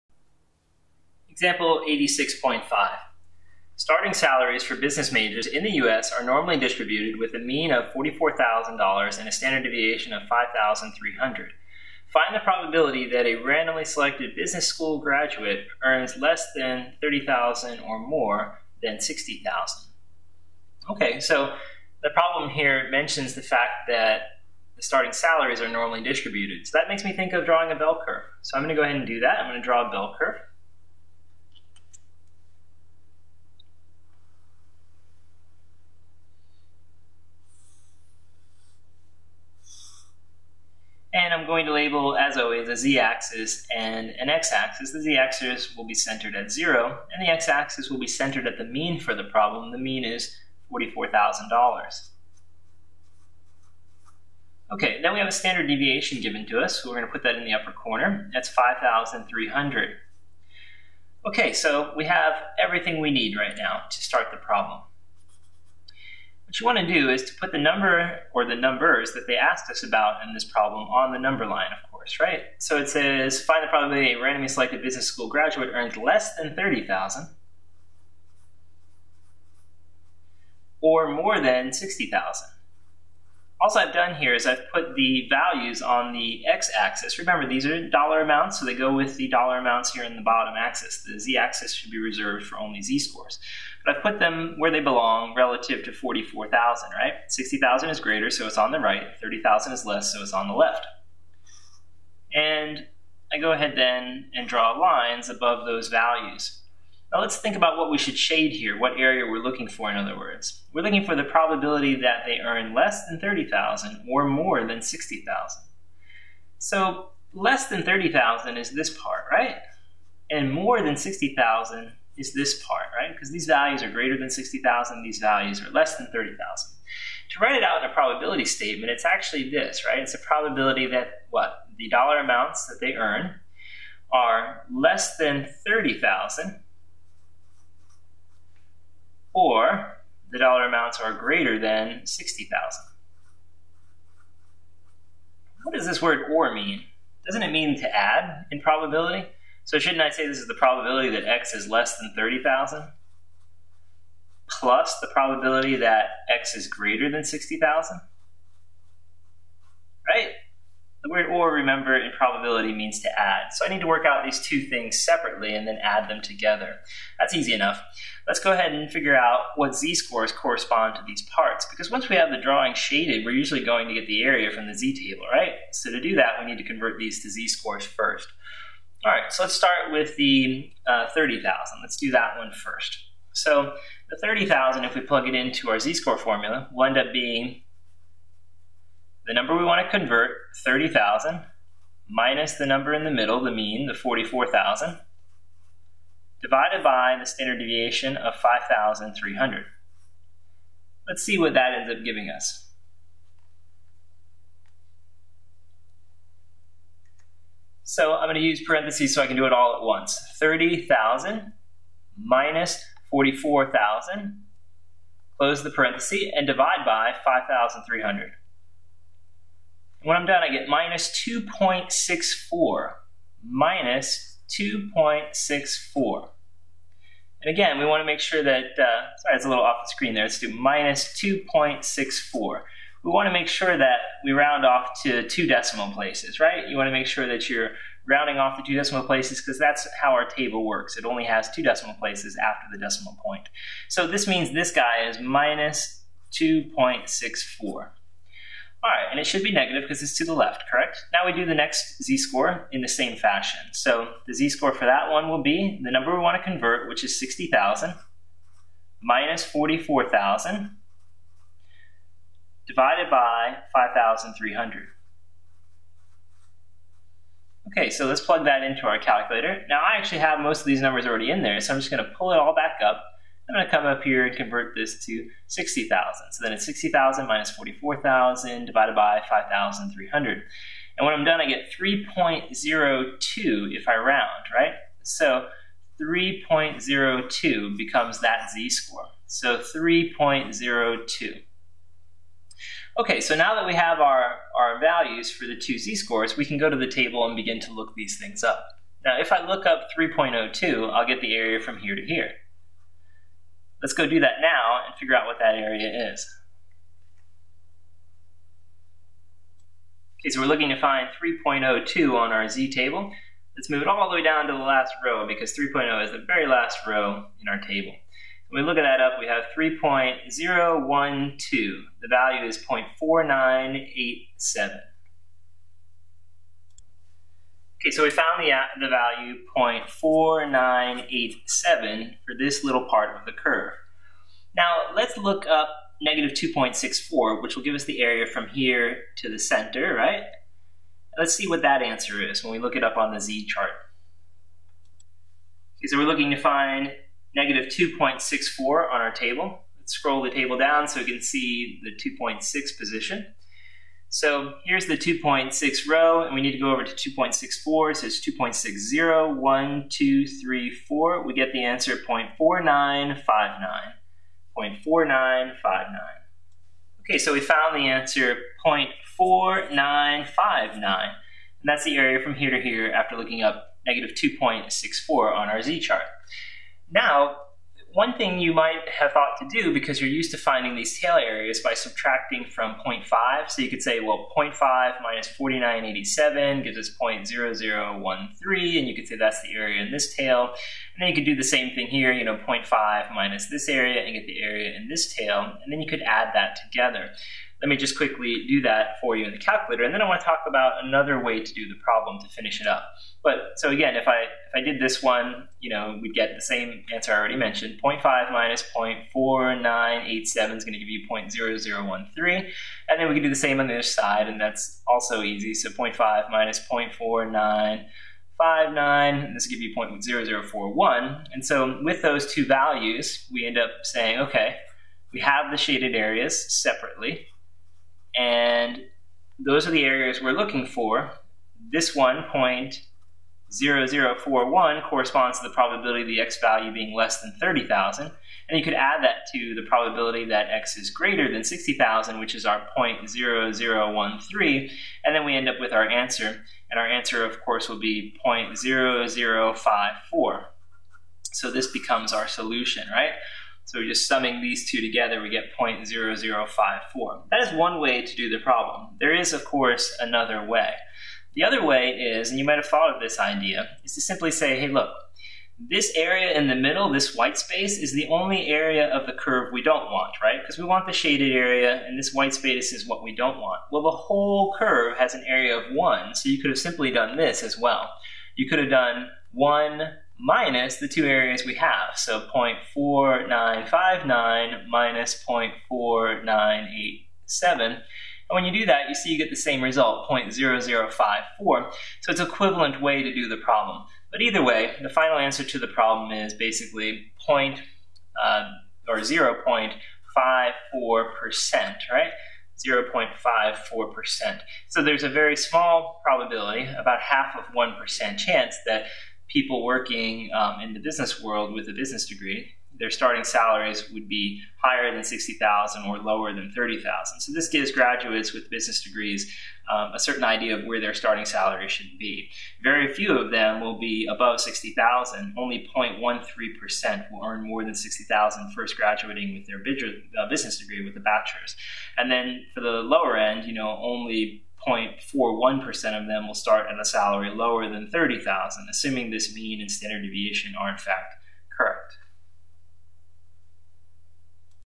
How to find probability (areas) under the normal curve (nonstandard normal), example 86.5
In this video, we demonstrate how to find an area (probability) under the normal curve using a typical z table.